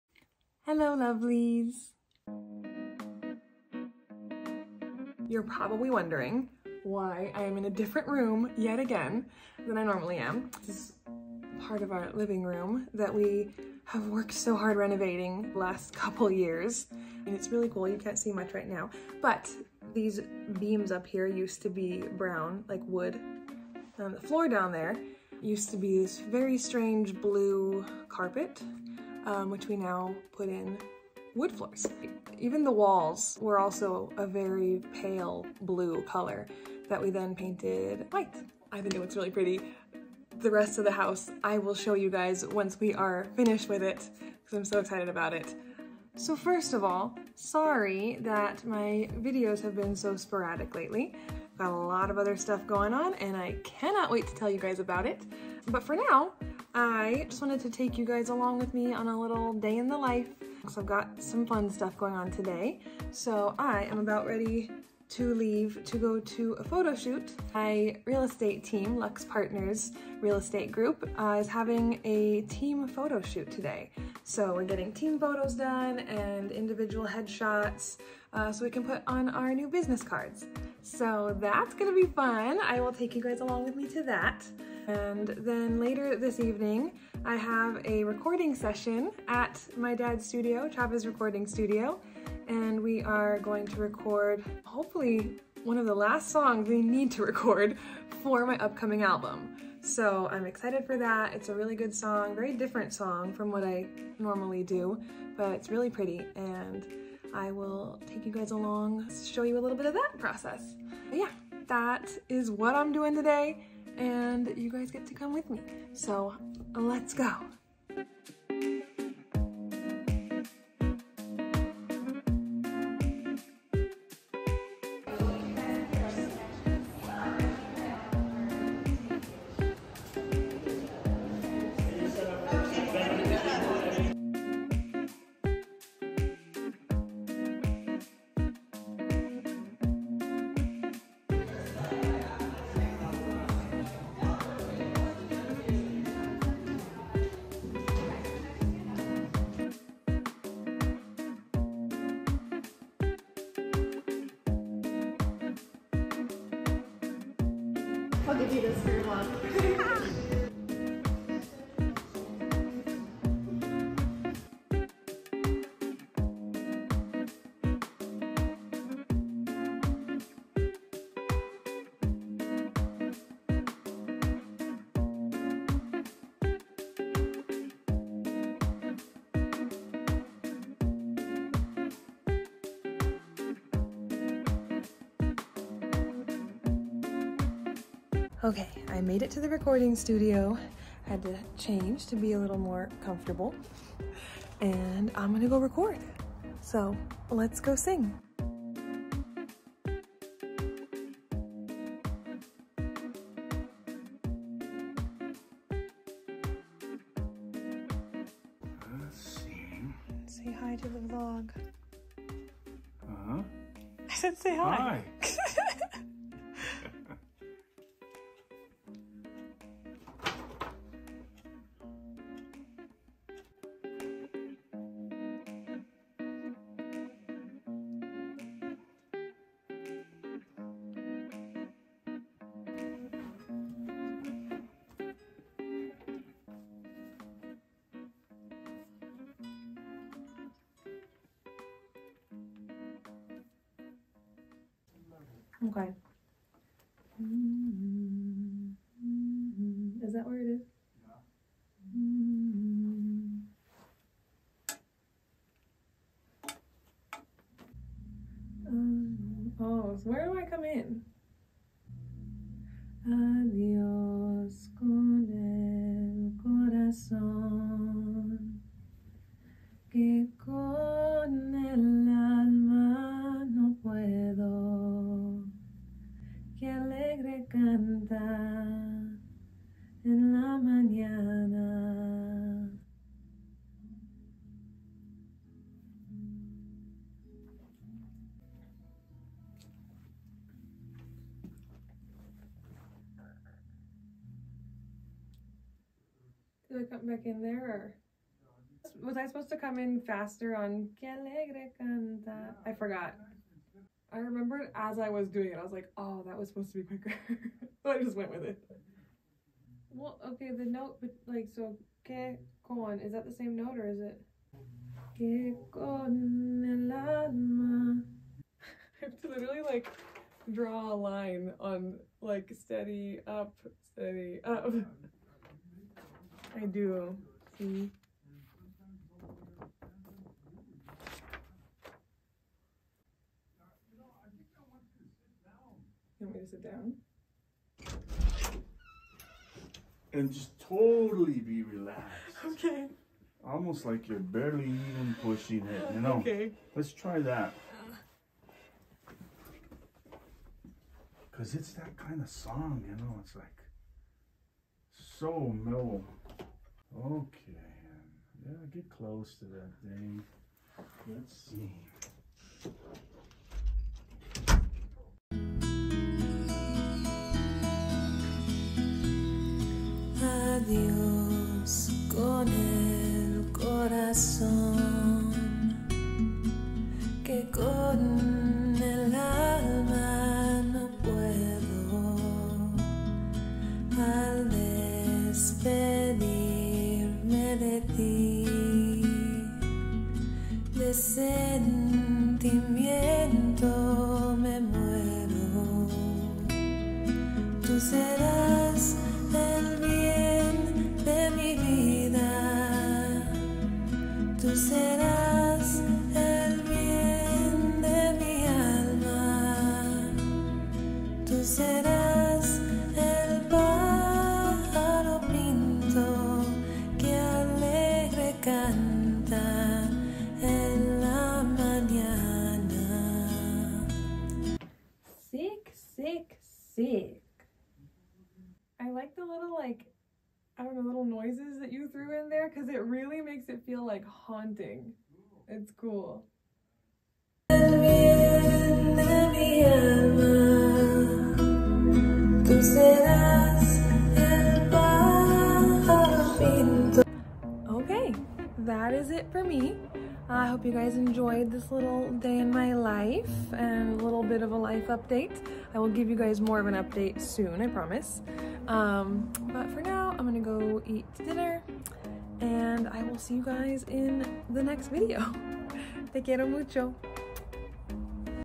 Realtor By Day, Singer By Night!
Come along with me as I start my day with real estate stuff and end my day with singing stuff! I also give a bit of a life update…more in-depth updates to come soon, though!